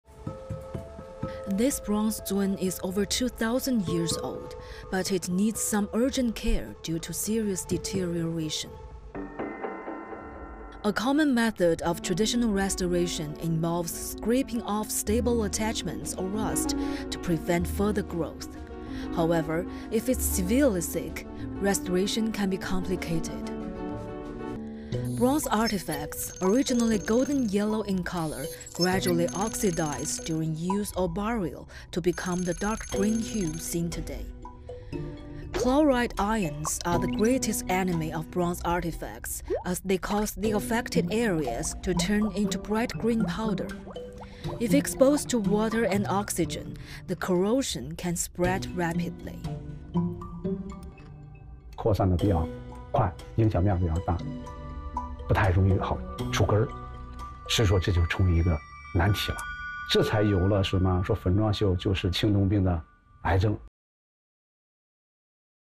Removing rust key to restoring bronze artifacts
Chloride ions are the greatest enemy of bronze artifacts since they cause the affected areas to turn into bright green powder. A common method of traditional restoration involves scraping off stable attachments or rust to prevent further growth.